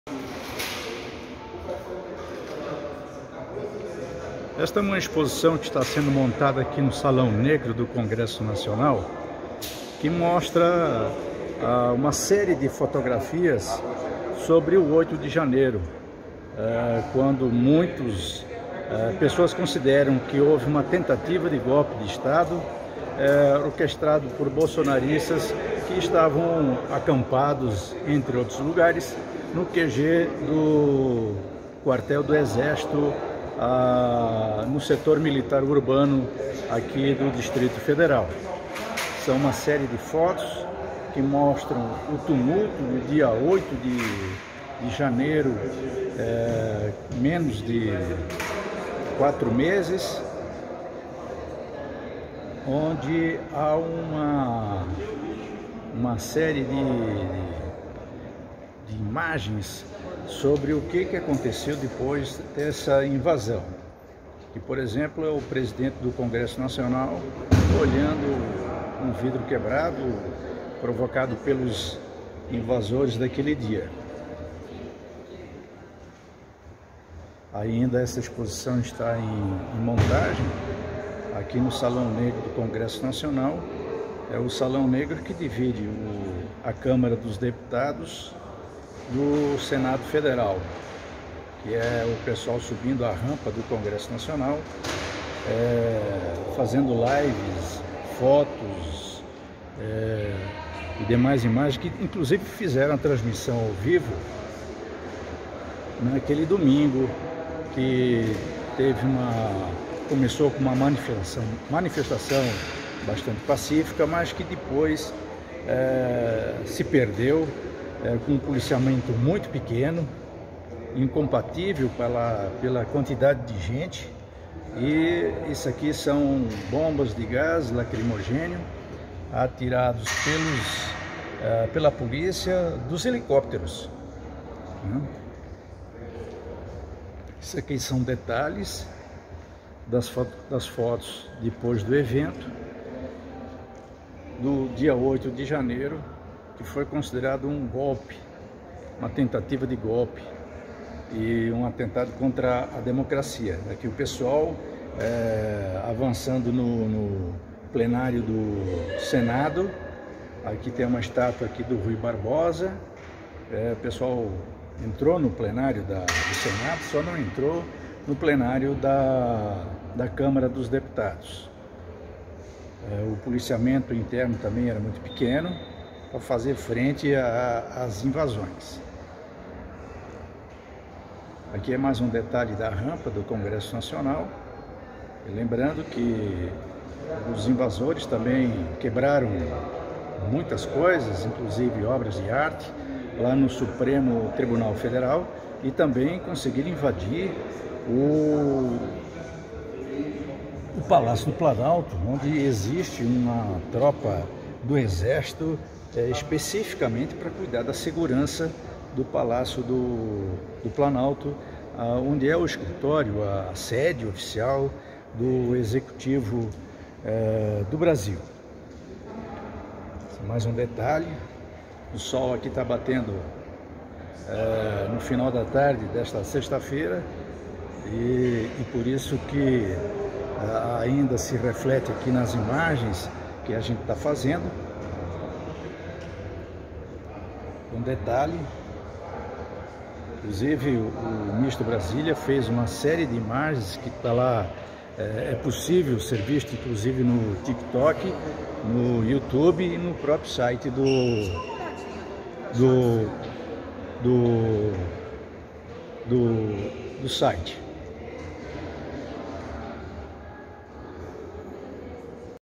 Exposição mostra imagens dos atos antidemocráticos do dia 8 de janeiro
Fotos da manifestação violenta estão numa exposição que está montada no Salão Negro do Congresso Nacional. O Senado Federal realiza, entre os dias 18 e 26 de abril. A mostra reúne 50 imagens do ataque ao Congresso Nacional capturadas pelos olhares e lentes dos fotojornalistas credenciados que cobrem o dia a dia do Parlamento, seja da imprensa externa ou dos veículos do Senado.